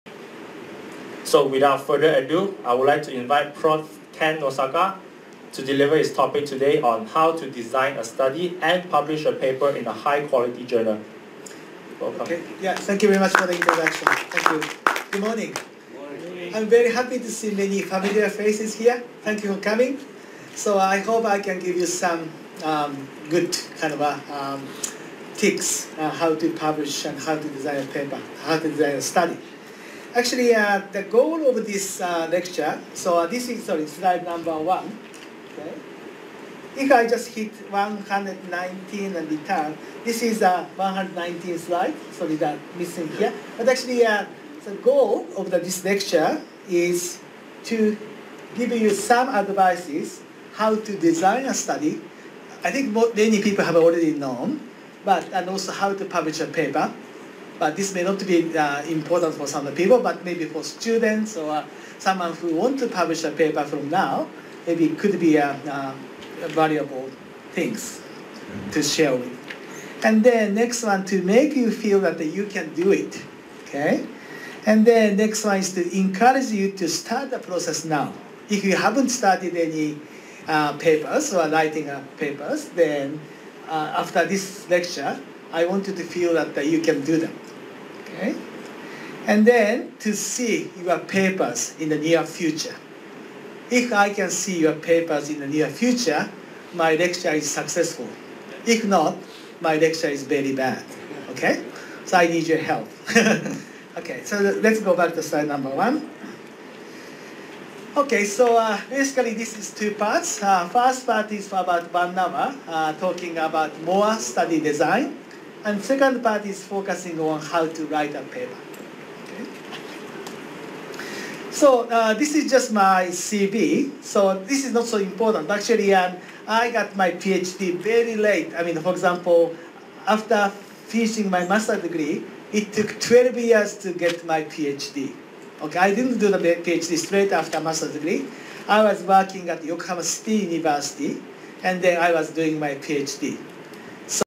HOW TO DESIGN A STUDY AND PUBLISH A PAPER IN A HIGH QUALITY JOURNAL : PART 1 HIGHLIGHT
ISN LECTURE SERIES HIGHLIGHT
TITLE: Part 1/2: How to Design a Study and Publish a Paper in a High-Quality Journal?
BY: Prof. Ken NosakaCenter for Exercise and Sports Science Research, School of Medical and Health Sciences, Edith Cowan University Australia